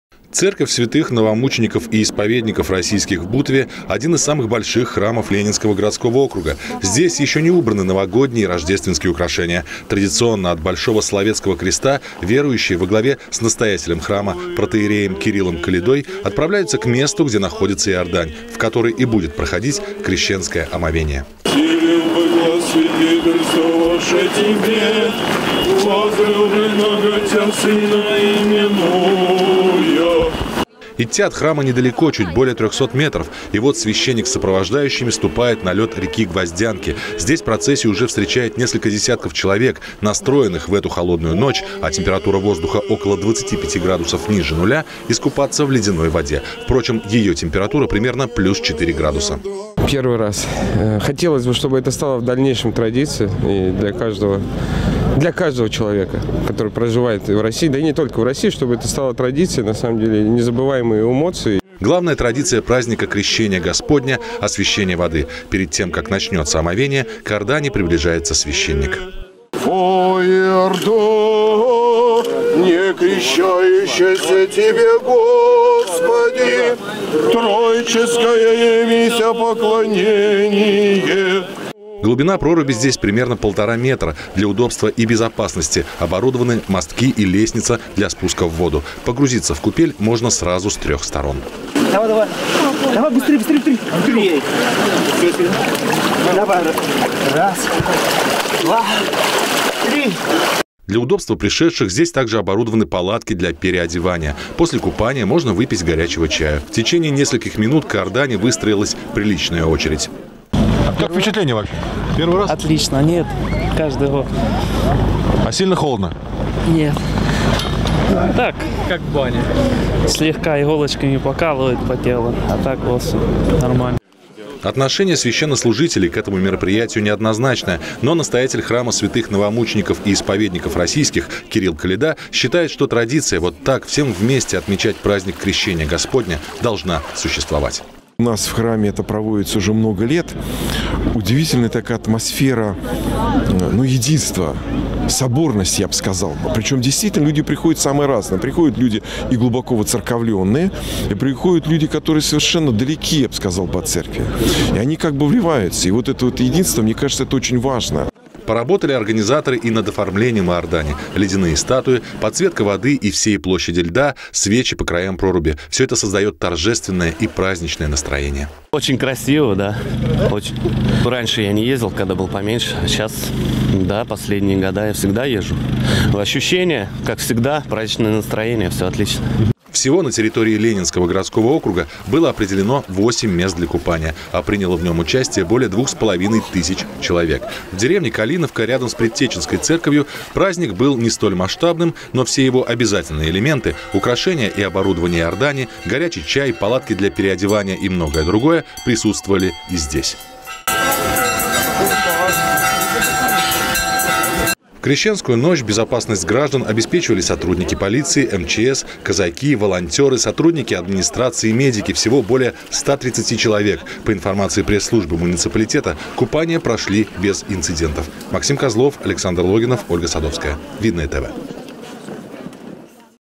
«Крещение Господне» - крещенский праздник в Ленинском городском округе
В ночь с 18-го на 19-е января, несмотря на настоящие крещенские морозы, около 4 тысяч верующих нашего муниципалитета приняли участие в праздничных мероприятиях, из них 2,5 тысячи совершили омовение в специально оборудованных купелях и иорданях.